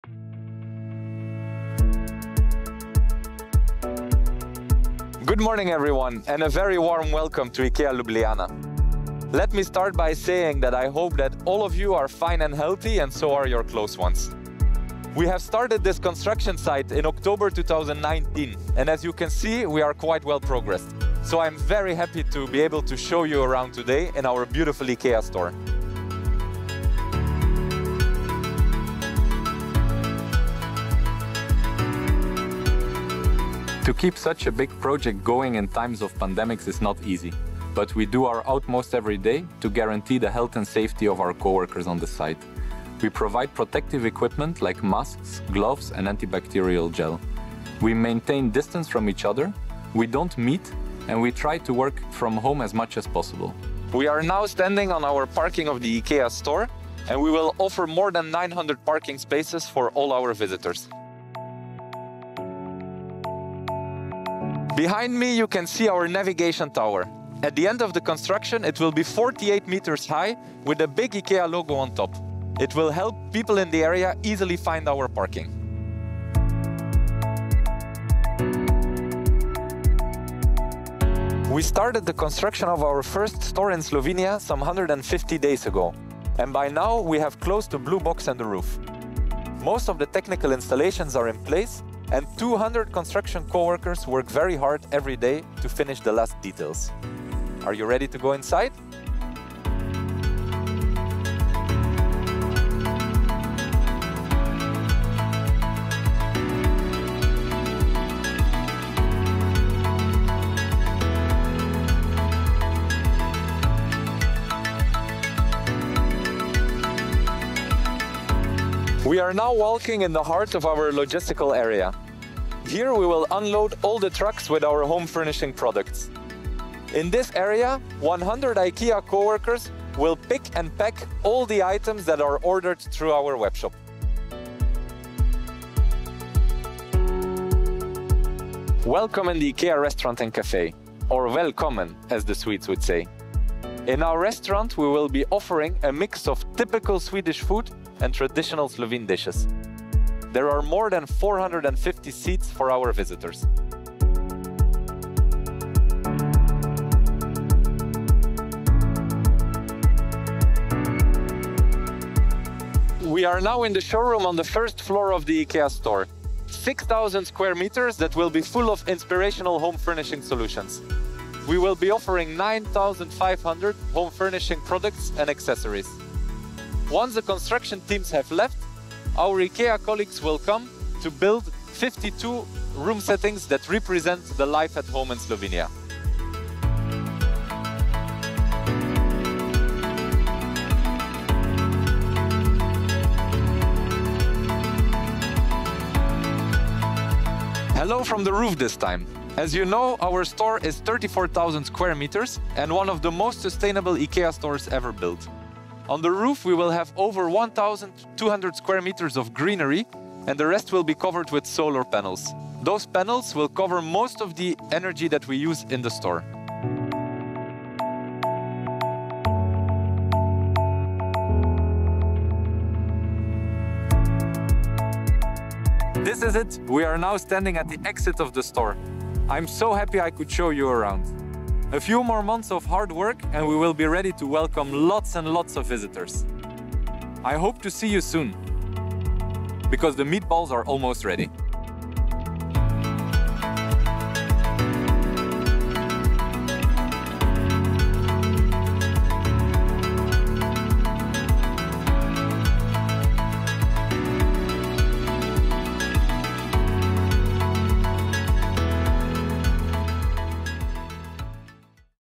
IKEA že pokrita s streho
Gradnja IKEE tudi v času epidemije koronavirusa napreduje po načrtih in že dobiva svojo pravo podobo.